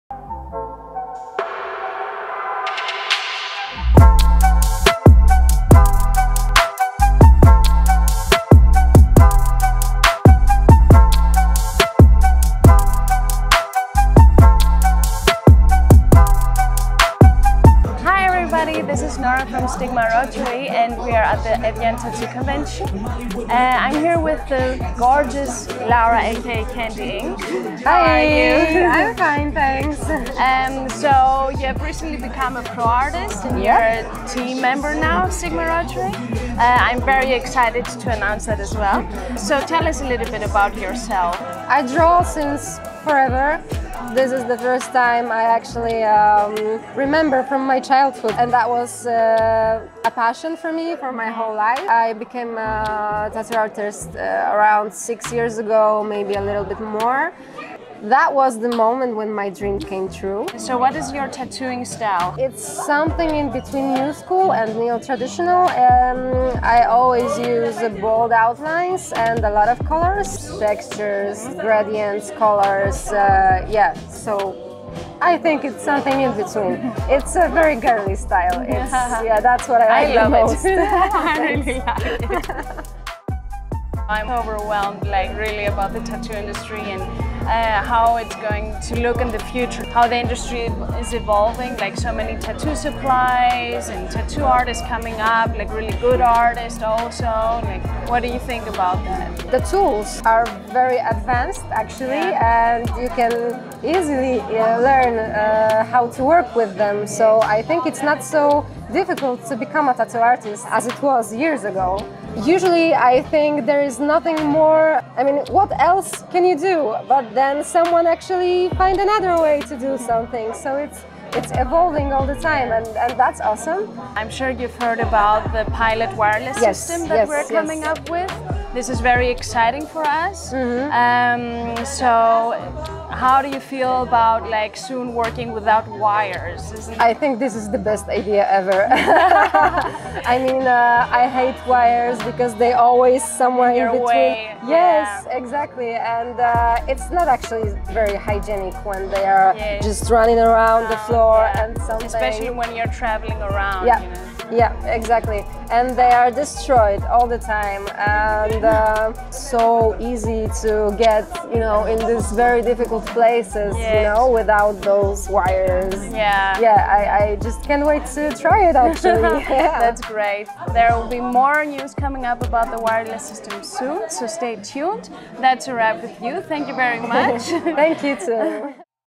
Laura Candy Ink Interview at Evian Tattoo Show 2019
A brief conversation with Laura Candy Ink unveils Laura's background as a tattoo artist and how she creates her neotraditional / newschool / girly tattooing style. We tap into subjects like: how do we see the future of tattooing? How do tattoo artists react to upcoming products? All that and more is covered in this brief chat between Nora and Laura Candy Ink.